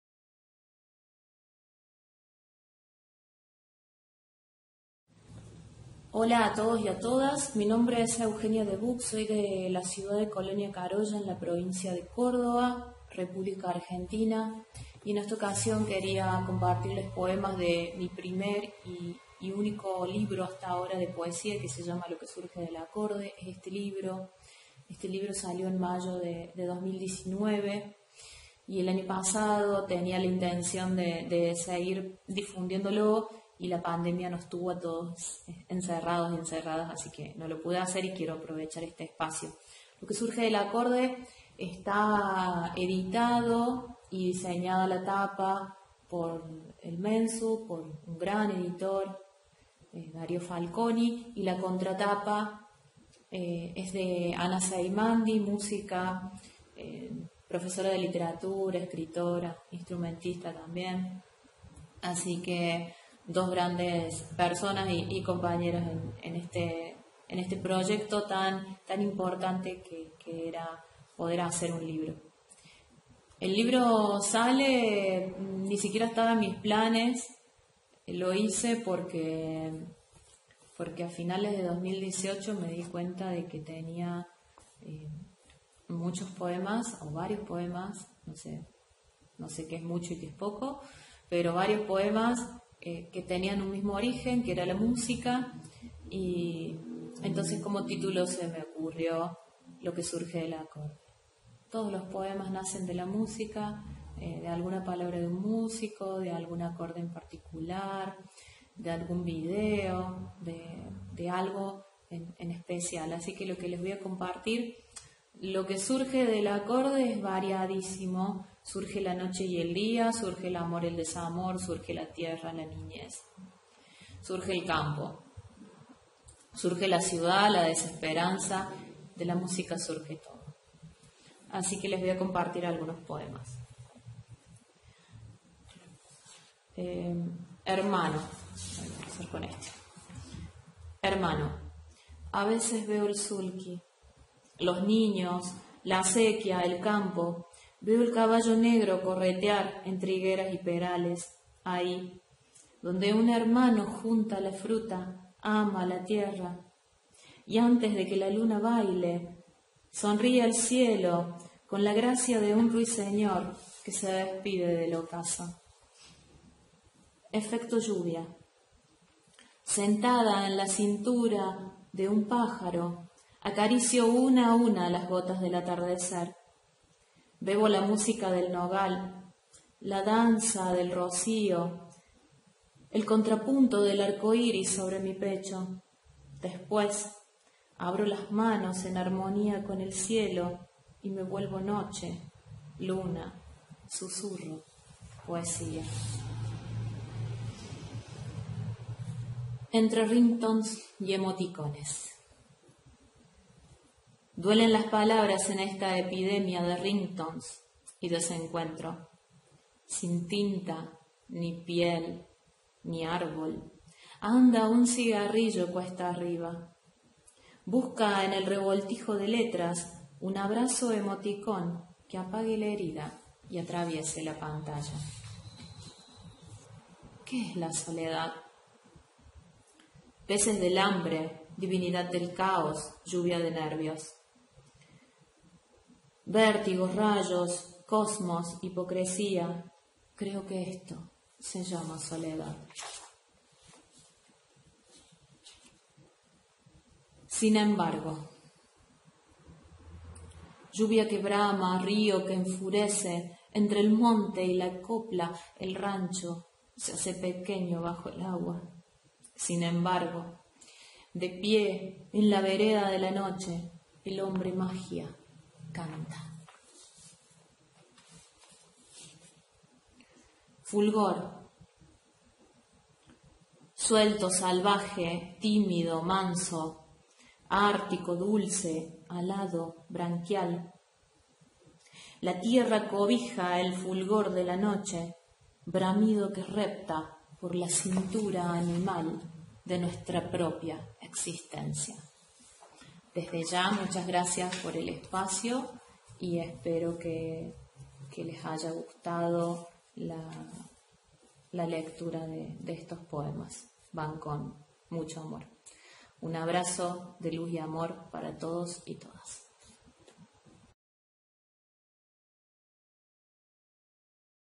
Presentación del libro: "Lo que surge del acorde" de Eugenia de Buck – Colonia Caroya - Argentina
Monte Hermoso, Buenos Aires, Argentina.